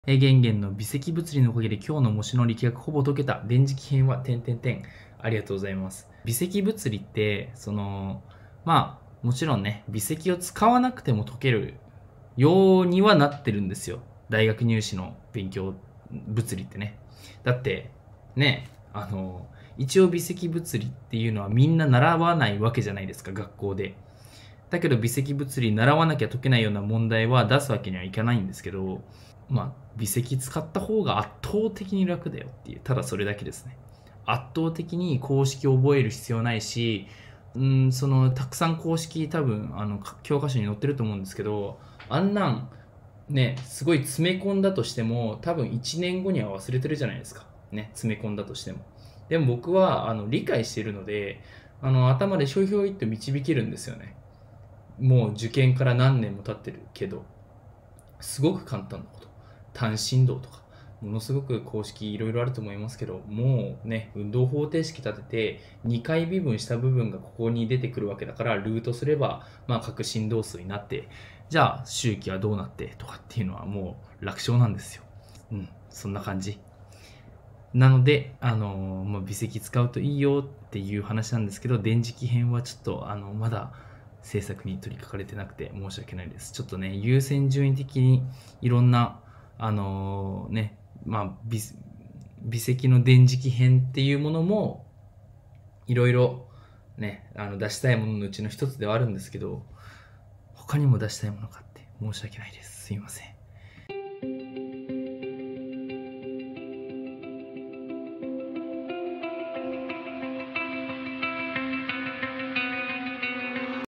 【河野玄斗】高校物理が苦手？圧倒的に楽になる勉強法教えます！【物理/勉強/受験】
0:00 物理
0:29 圧倒的に楽
1:21 電磁気編はまだ・・

東大医学部卒・司法試験合格・頭脳王3回優勝の河野玄斗さんの切り抜きチャンネル。
勉強・受験・模試・試験・資格など自分磨きにお役だてください。

関連動画

※Stardy様の正式な許可を得て、動画投稿を行っています。